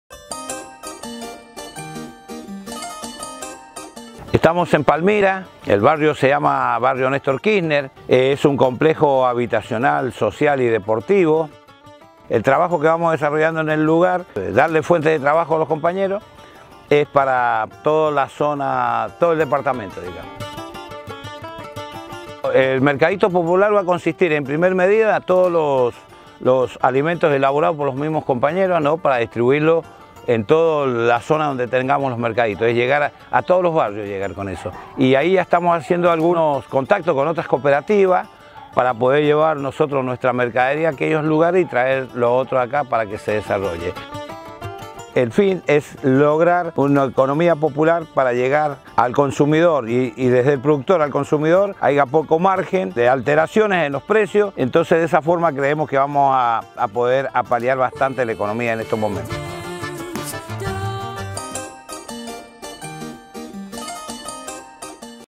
ECONOMÍA POPULAR: MERCADITO DE PALMIRA, SAN MARTÍN
El Mercadito Popular para una Mejor Alimentación funciona en el Barrio Néstor Kirchner de Palmira, en el Departamento de San Martín. Entre sus productos se ofrecen los que hacen las y los vecinos, y se intercambian otros con cooperativas de la provincia. Además, promueve el comercio justo entre productores y consumidores, para así hacer crecer el sector de la economía social y popular. Aquí, El Mercadito de Palmira en el marco del programa Desafíos, realizado gracias a los proyectos Mauricio López.